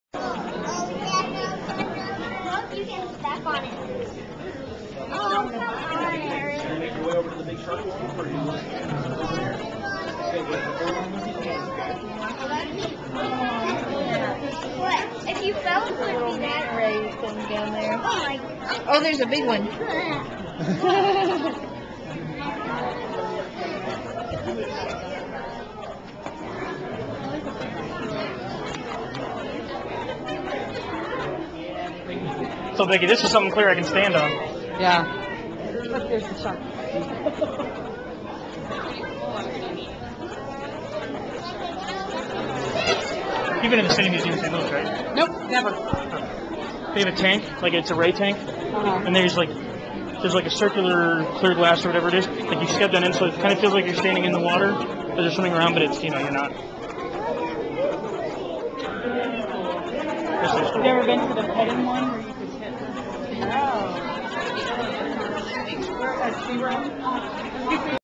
Standing on Stingrays
Clear tank at Shedd Aquarium lets you walk above and watch stingrays. Ignore everything after the :29 mark, I forgot to stop recording and you can't see anything anyway.